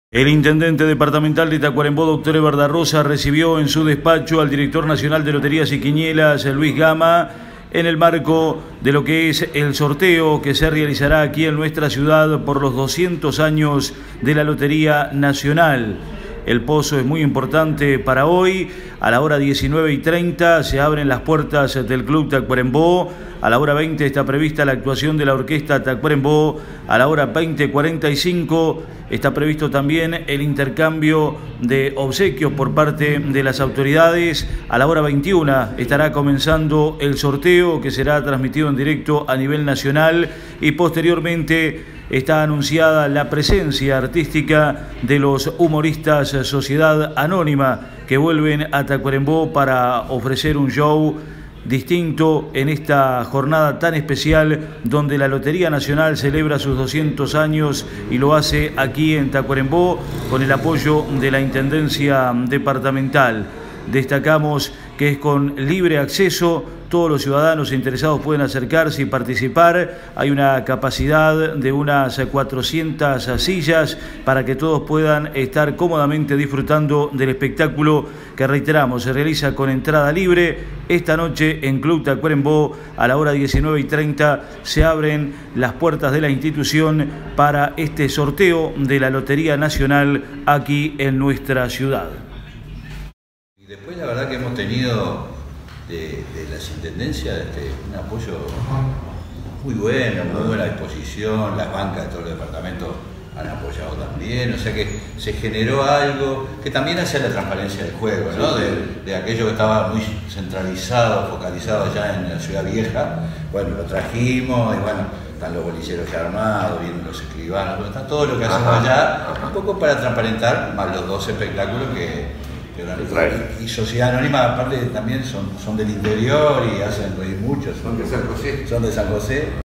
200 años de la Lotería Nacional
El sorteo se realiza hoy viernes 20 en Club Tacuarembó.